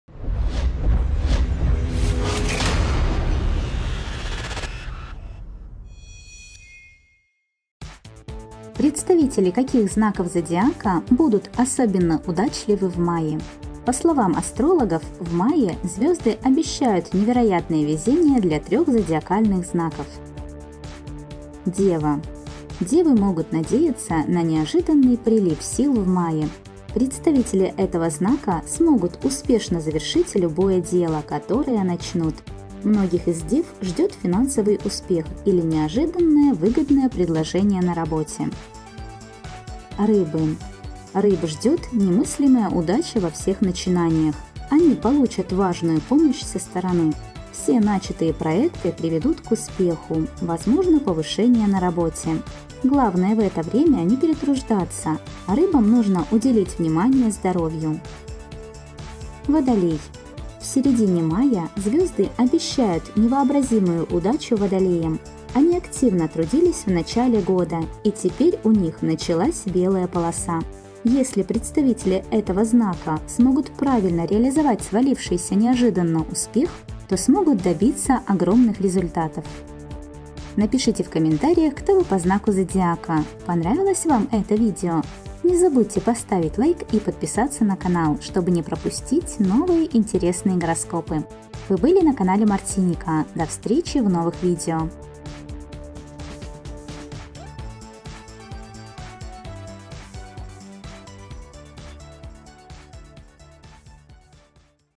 Сумасшедший УСПЕХ и крутые виражи СУДЬБЫ ждут знаки зодиака в МАЕ 2022
Представители каких знаков зодиака будут особенно удачливы в мае
По словам астрологов, в мае звезды обещают невероятное везение для трех зодиакальных знаков.

Напишите в комментариях кто вы по знаку зодиака. Понравилось вам это видео?